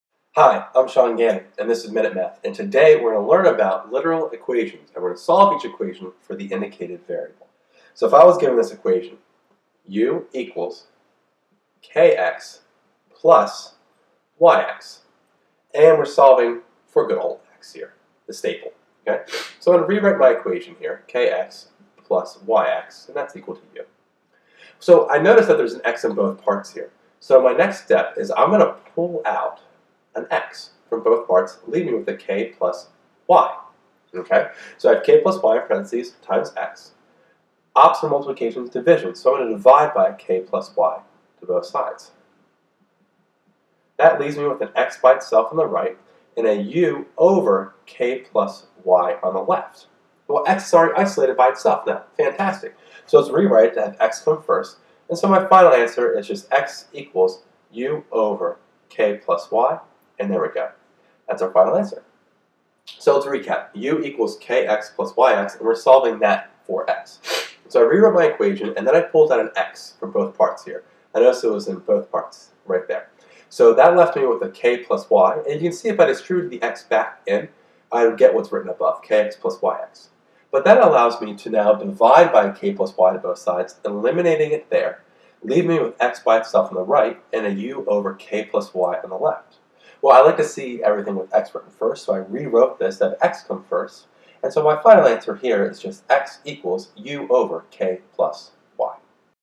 u=kx+yx, solve for x
In this math video lesson I solve the equation u=kx+yx , for x. This is a useful skill for students who are in Algebra and will help them to better understand Literal Equations.